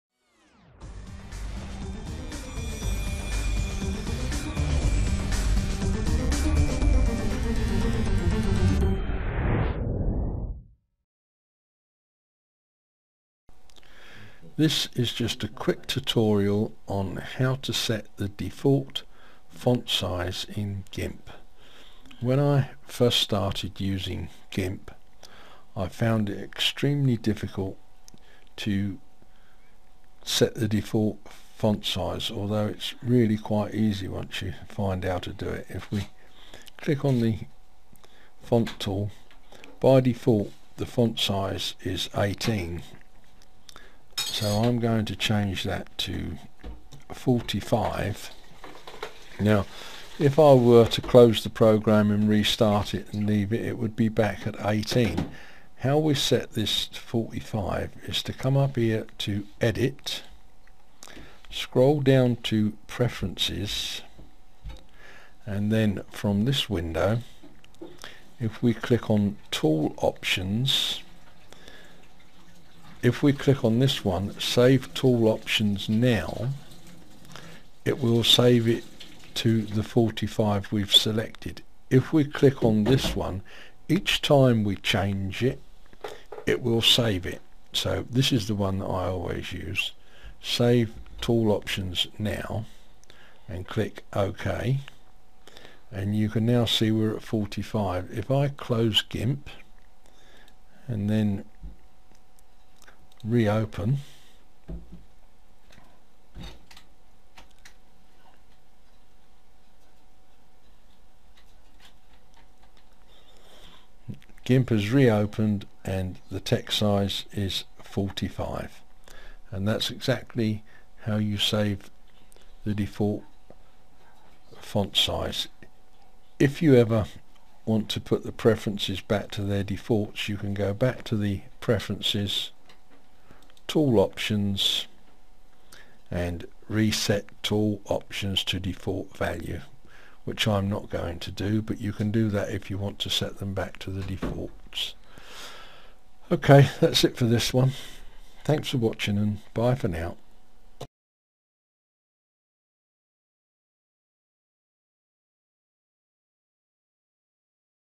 Gimp 2.8 Set Default Text Size /GimpText Tool - EASY / How To
SET DEFAULT TEXT SIZE GIMP

In this video, I will demonstrate how to set the Default Text Size in Gimp 2.8 text tool.
To illustrate this, I'm useing Gimp 2.8, in single-window mode.